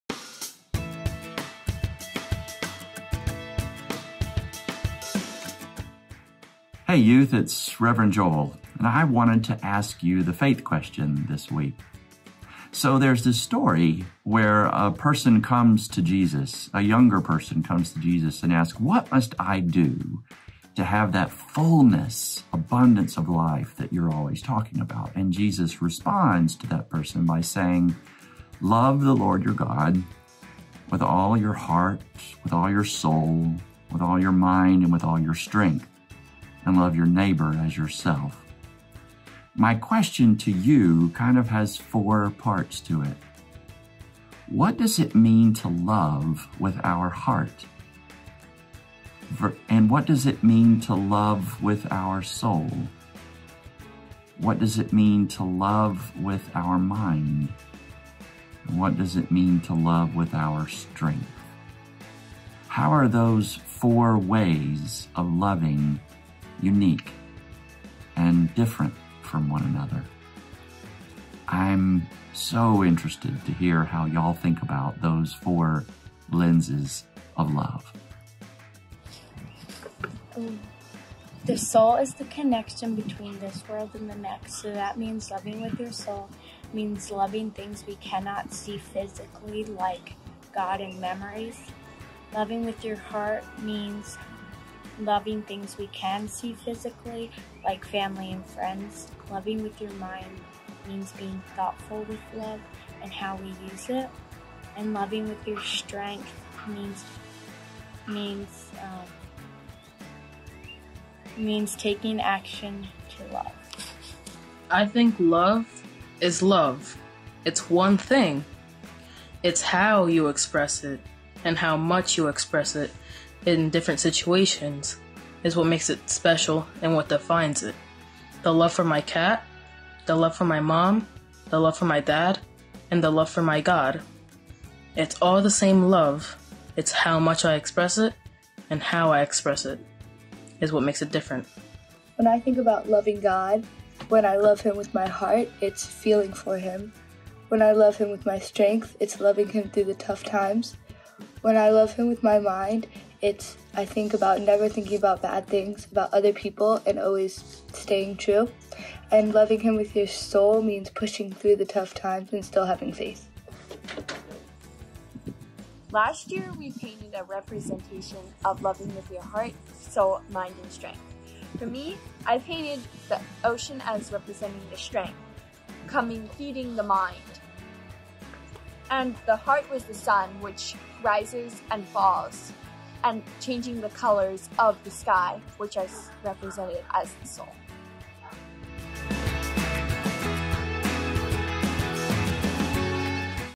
Faith Qs for Youth - How is love different through our heart, spirit, mind, or strength?
Rev. Joel asks our youth about the four different lenses of love Jesus lifted up when he taught us to love God, our neighbors, and ourselves through our heart, spirit, mind, and strength. How are those lenses different from one another and unique expressions of the same love?